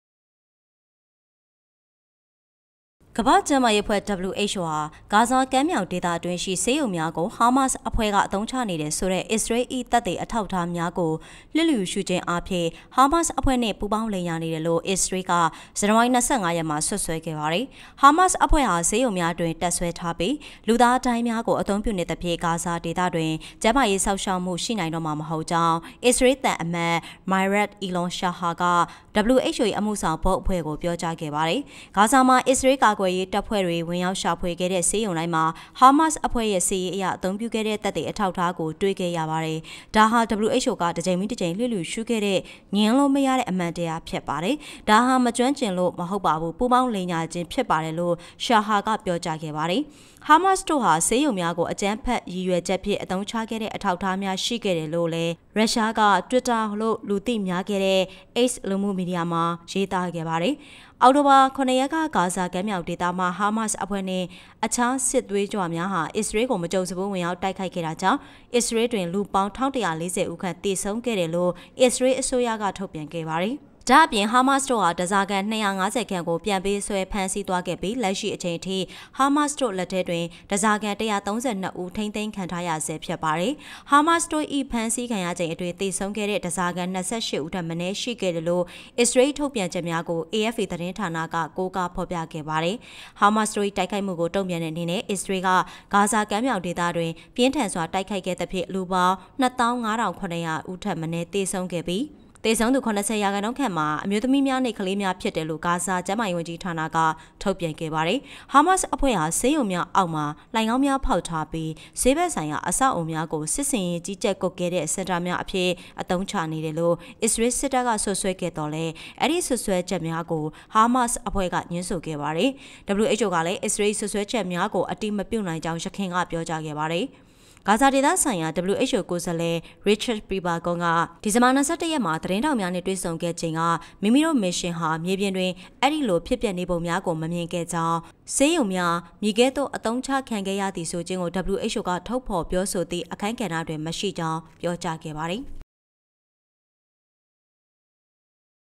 WHO သည် ဟားမာ့စ်အဖွဲ့နှင့် ပူးပေါင်းလိမ်ညာနေကြောင်း အစ္စရေး စွပ်စွဲ
ကမ္ဘာ့ကျန်းမာရေးအဖွဲ့(WHO)သည် ဂါဇာကမ်းမြောင်ဒေသအတွင်းရှိ ဆေးရုံများကို ဟားမာ့စ်အဖွဲ့က အသုံးချနေသည်ဆိုသည့် အစ္စရေး၏ သက်သေအထောက်အထားများကို လျစ်လျူရှုခြင်းအားဖြင့် ဟားမာ့စ် အဖွဲ့နှင့် ပူးပေါင်းလိမ်ညာနေသည်ဟု အစ္စရေးက ဇန်နဝါရီ ၂၅ ရက်တွင် စွပ်စွဲခဲ့သည်။